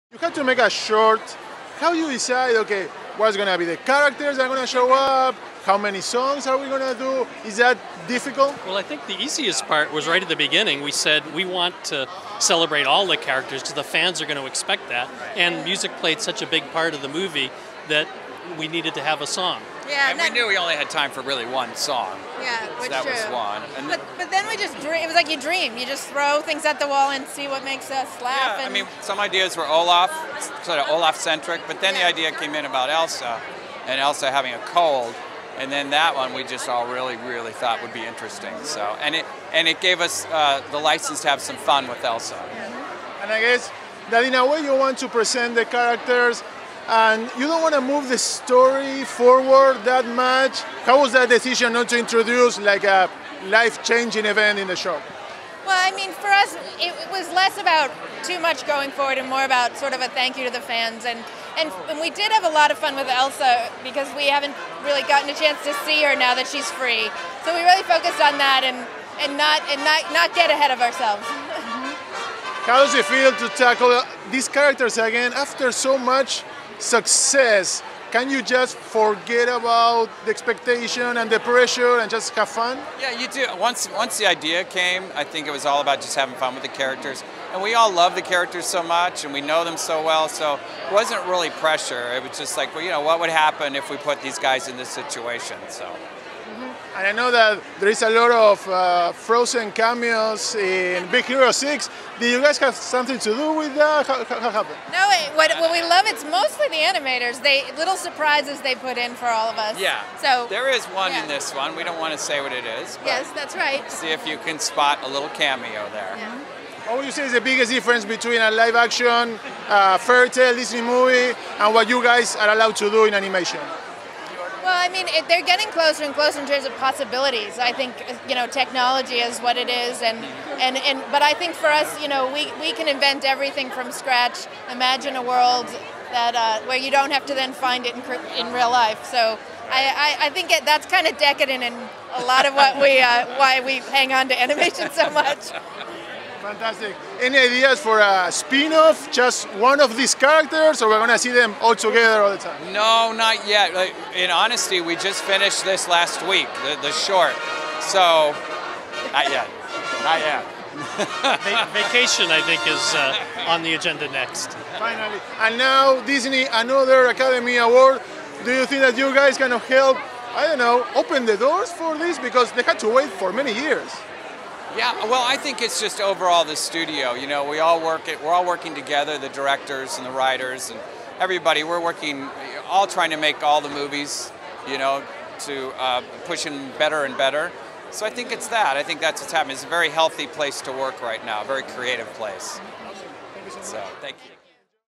FROZEN FEVER Creators at CINDERELLA Premiere
At the CINDIRELLA premiere I had the chance to talk to the creators of FROZEN FEVER, producer Peter Del Vecho, and directors Chris Buck and Jennifer Lee (FROZEN).
Watch our exclusive interview where the trio talks about the biggest challenges and pleasures of doing a new story with the main characters and cast members from the Academy Award Winner animated Disney film